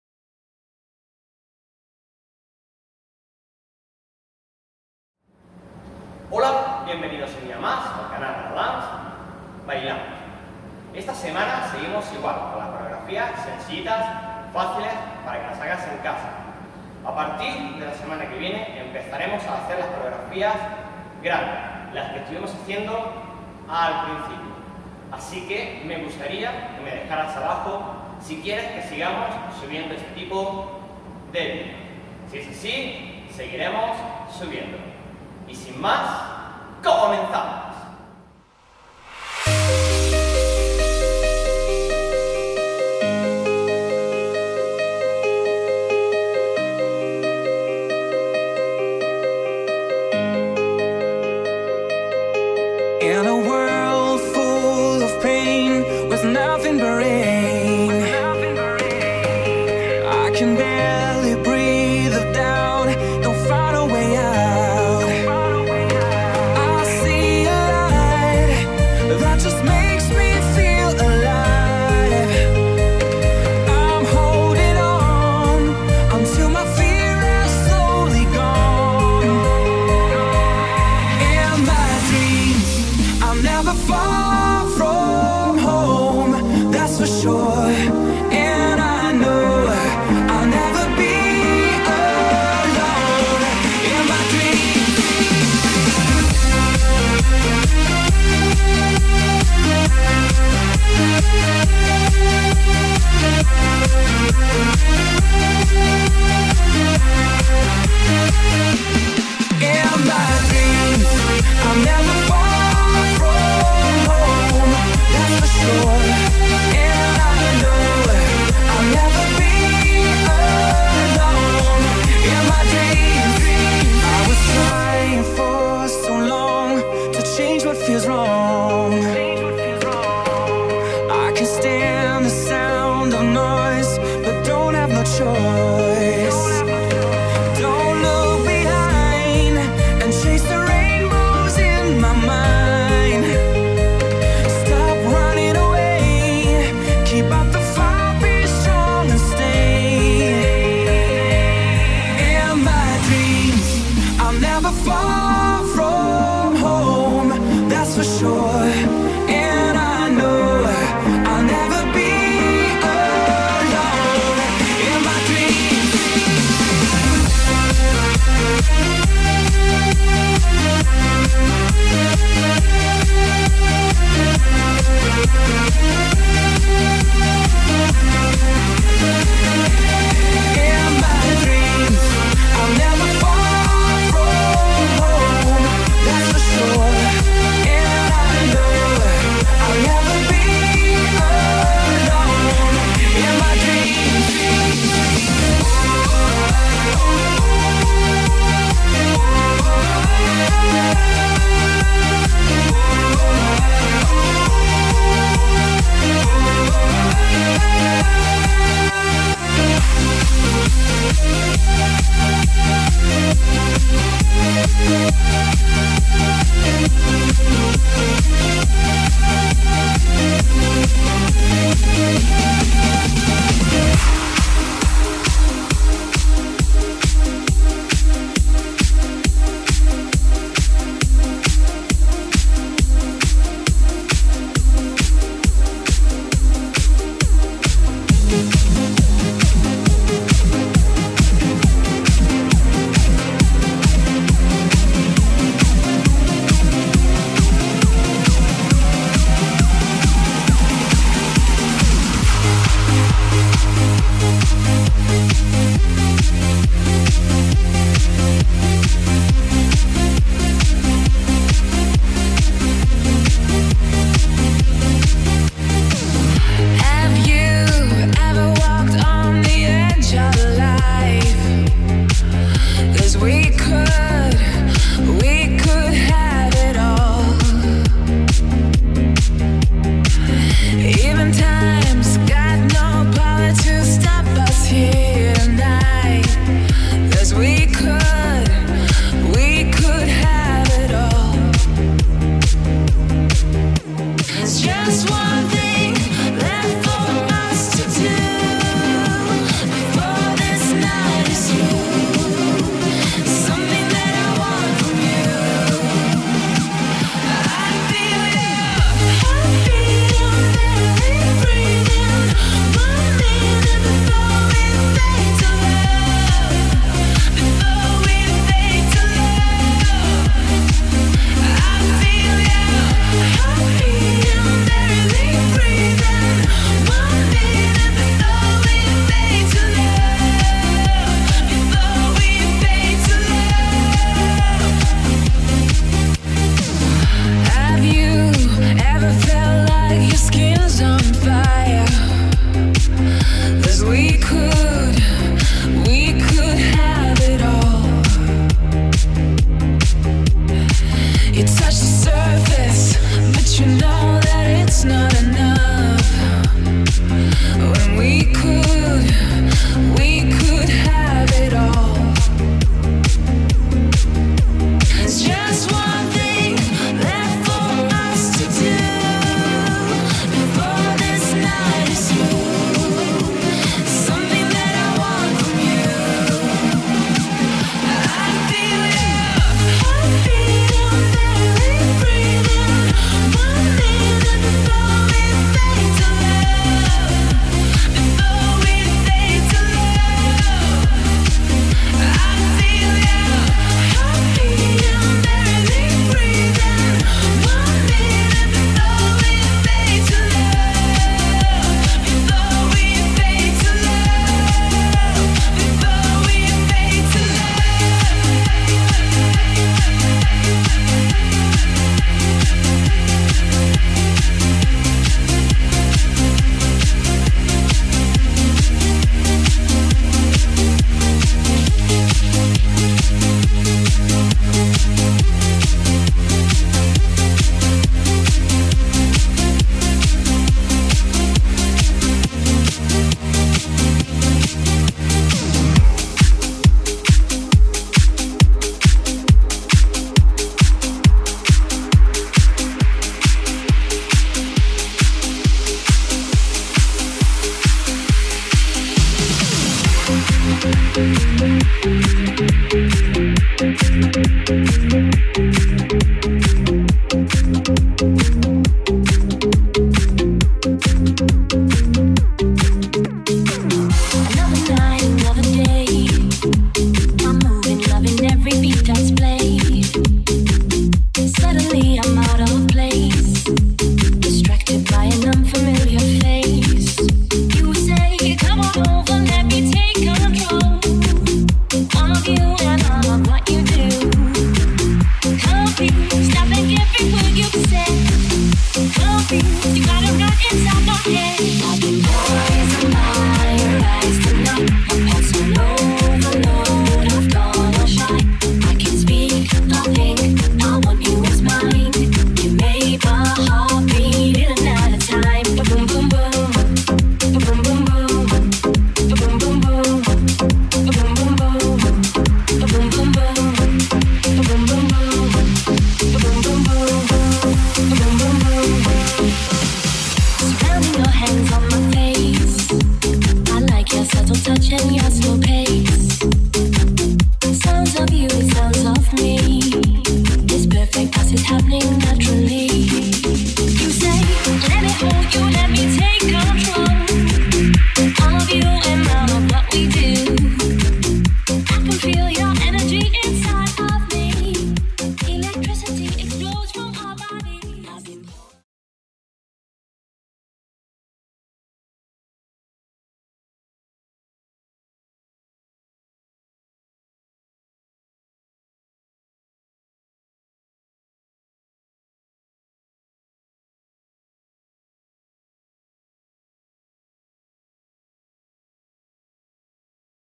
Aerodance"Mini clase aerobic"#6
Un video más para esta semana,seguro que ya sois unas expertas bailarinas en casa,por eso me gustaria saber si quereis que siga subiendo este tipo de videos,ya que apartir de la semana que viene empezamos con las coreografias mas complejas,dejame tu opinion al respecto.
Gracias por seguirnos,deja tus comentarios y tu like y si aun no estas suscrito..¡¡a que esperas!!..un saludo😁😁